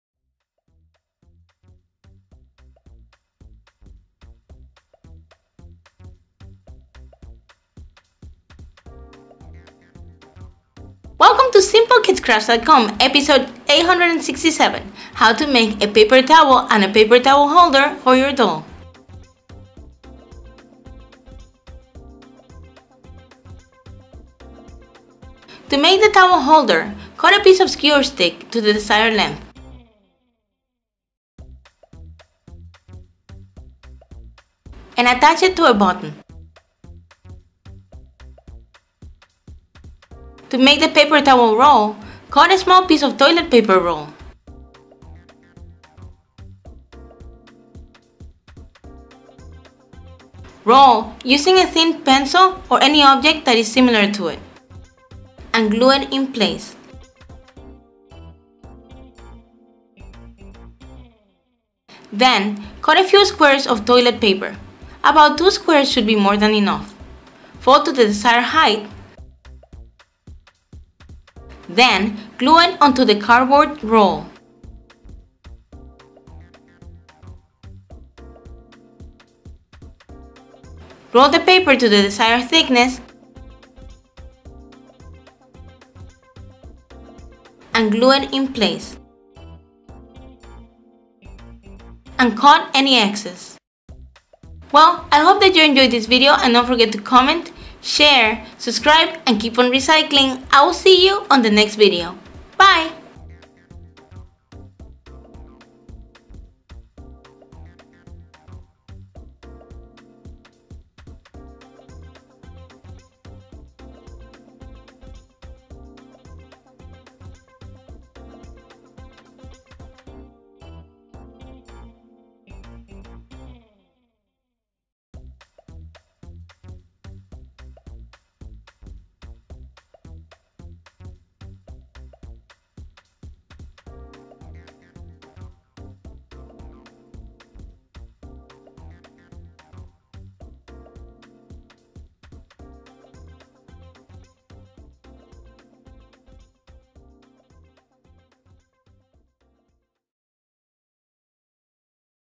How to make a paper towel and a paper towel holder - simplekidscrafts - simplekidscrafts
How to make a paper towel and  a paper towel holder for your doll - Easy Doll Crafts

XOXOXO,
Merve